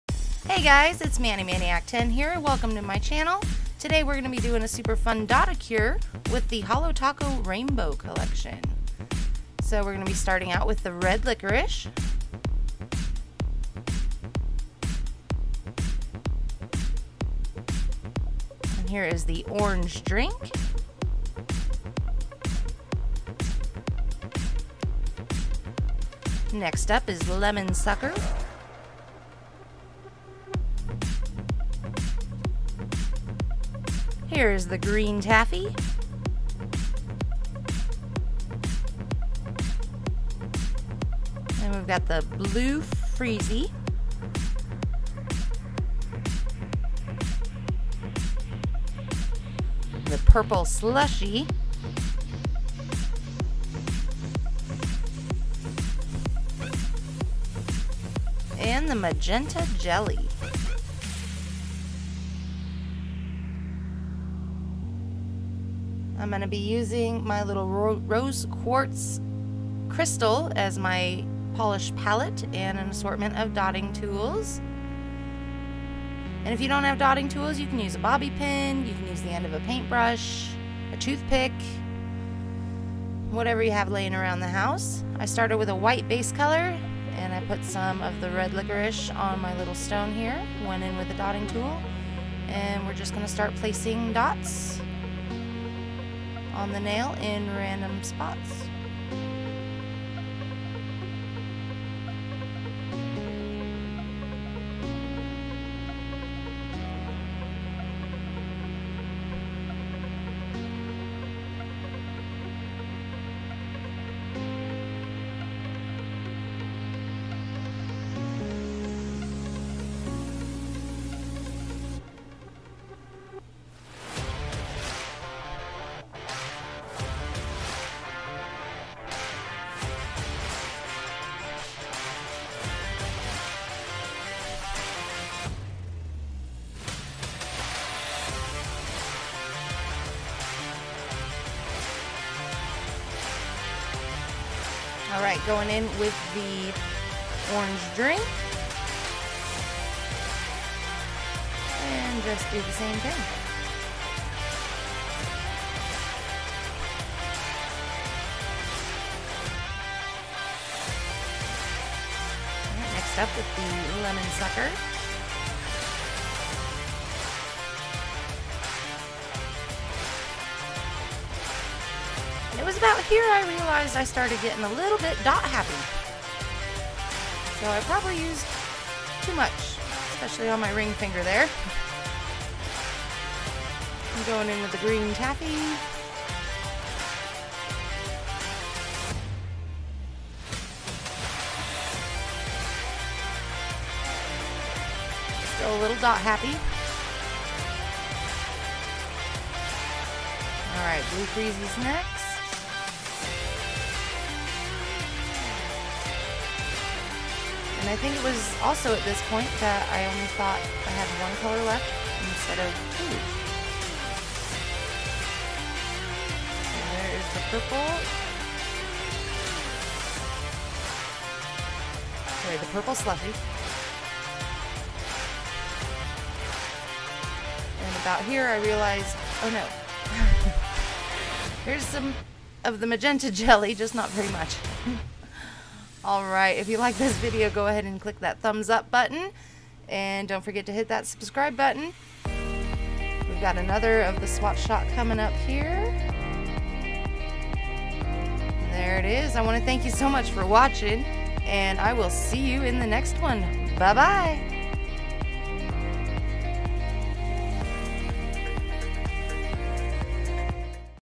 Holo Taco Rainbow Dotticure!!
What's up everybody? Today we are doing a rainbow dotticure using the Holo Taco Rainbow Collection!
I used Holo Taco
Red Liquorice, Orange Drink, Lemon Sucker, Green Taffy, Blue Freezie, Purple Slushie, and Magenta Jelly.
TheGel White
Salon Perfect Crystal Clear Top Coat
Maniology Rose Quartz Palate Stone